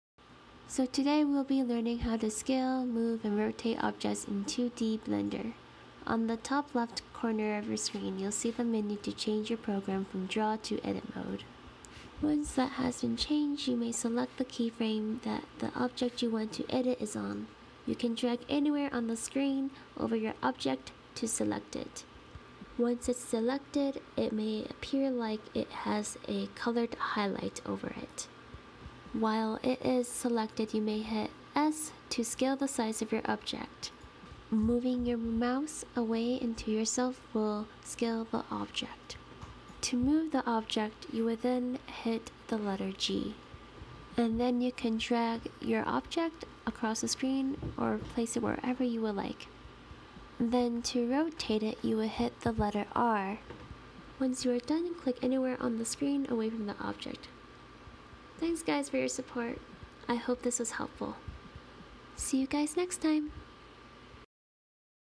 How to Resize, Move, Rotate in 2D Animation Blender
This is a quick tutorial about how to resize, move, and rotate your object/characters in 2d animation on Blender.